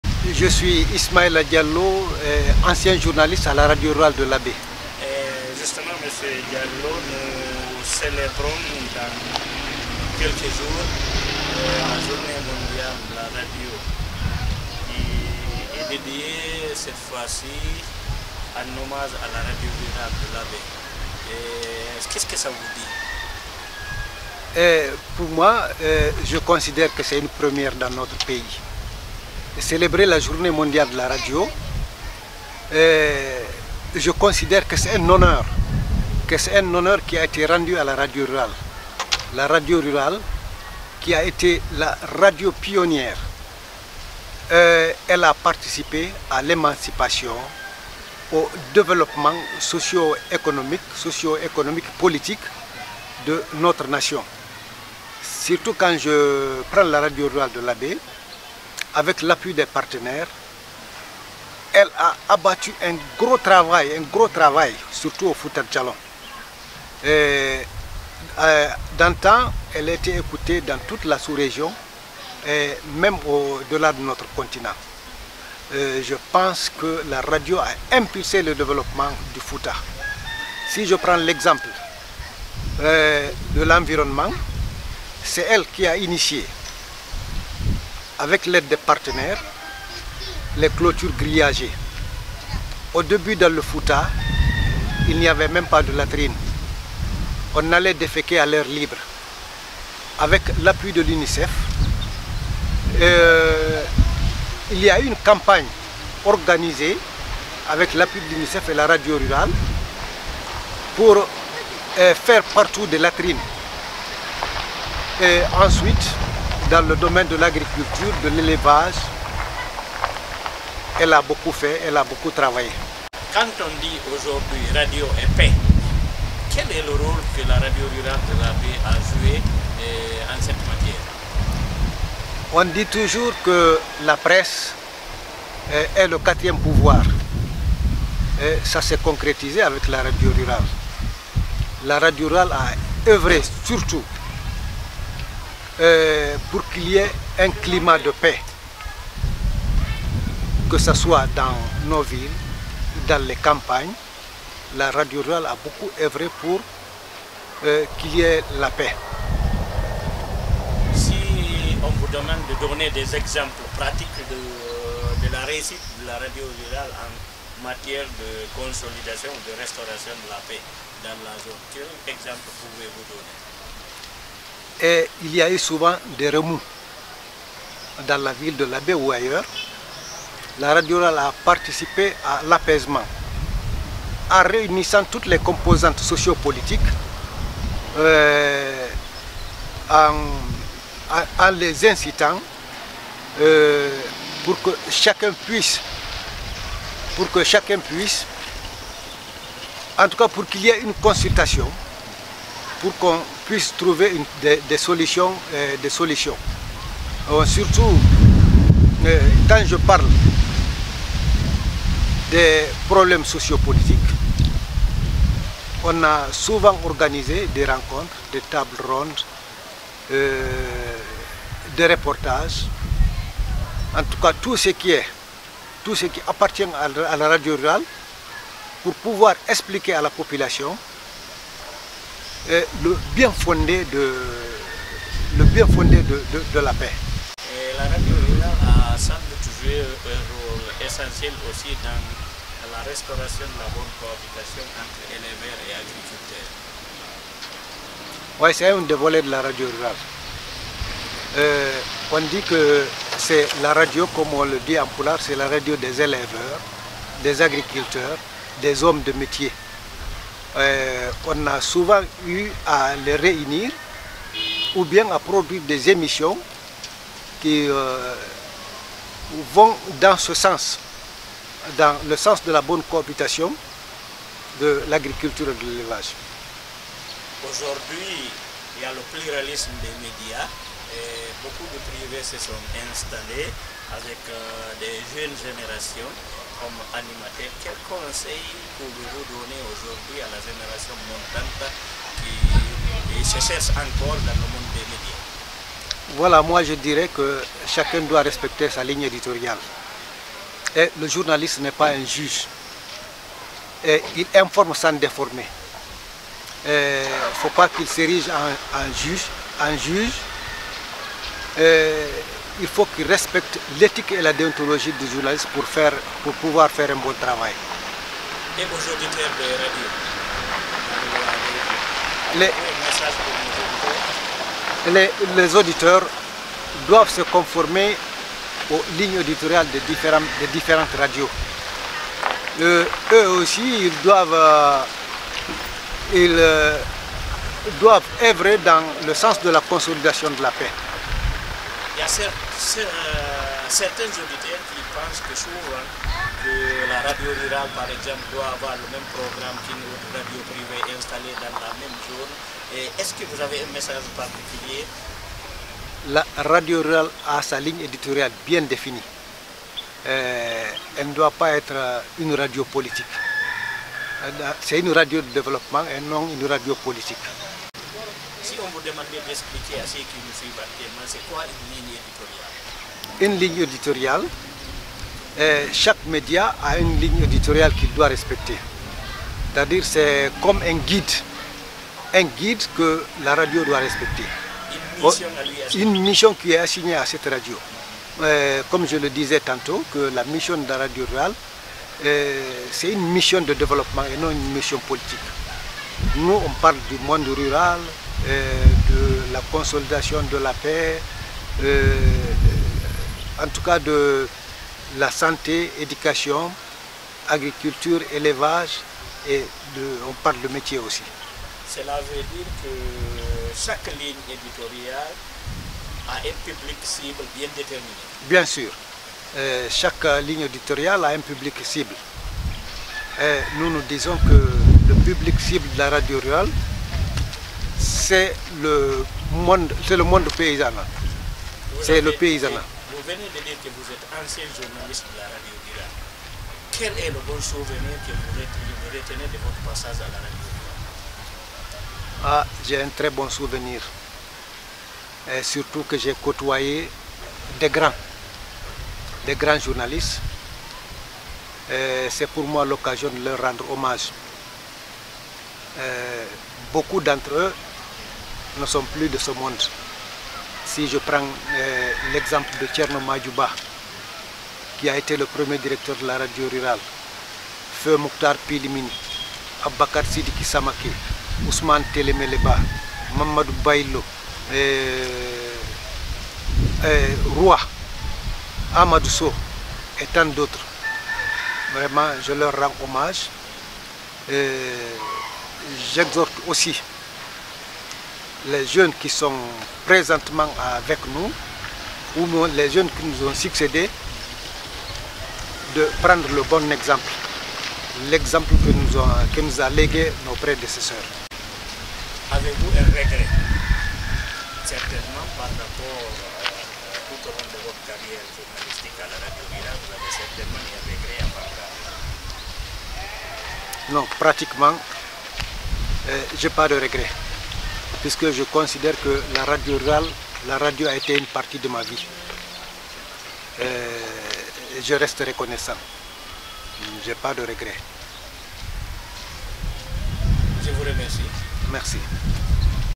Journée mondiale de la radio à Labé : les souvenirs de Diallo Ismaïla Mosquée, ancien journaliste
En prélude à la célébration de la journée mondiale de la radio prévue en différé à Labé pour le samedi, 25 février 2023, en hommage à la station régionale de la radio rurale de la Moyenne Guinée, notre confrère et doyen Ismaïla Mosquée Diallo, ancien animateur de ce premier médium de proximité en province a confié à Guineematin.com ses bons souvenirs du rôle joué par son organe de presse dans le maintien et la consolidation de la paix dans la région.

Ismaïla Mosquée Diallo a commencé, d’abord, par se féliciter de la décision des autorités du département à organiser cette cérémonie commémorative de la journée mondiale de la radio en hommage à la radio rurale de Labé.

Il est, ensuite, largement revenu sur le rôle de premier plan joué par la station régionale de la radio rurale de la Moyenne Guinée dans le maintien et la consolidation de la paix dans la région. Sans oublier de rendre un hommage à titre posthume à ses ainés qui l’ont reçu à la radio rurale de Labé comme le premier chef de la station, feu Thierno Maadjou Bah et ses proches collaborateurs feu Aboubakar Sidiki Samaké, feu Mouctar Pilimini Diallo, feu Mamadou Baïlo Diallo, feu Amadou Sow, feu Mamadou Bhoye Barry dit Colonel, feu Alpha Mamadou Baldé dit Roi, le doyen Boubacar Dieng et Hadja Aïssatou Bah qui font actuellement valoir leur droit à la retraite.

Après, il a prodiguer d’utiles conseils aux générations montantes qui animent actuellement la radio rurale de Labé et les radios privées locales sur la nécessité de respecter scrupuleusement les règles d’éthique et de déontologie du métier.

Votre quotidien en ligne Guineematin.com vous invite à suivre l’intégralité de cet entretien dans la vidéo ci-dessous.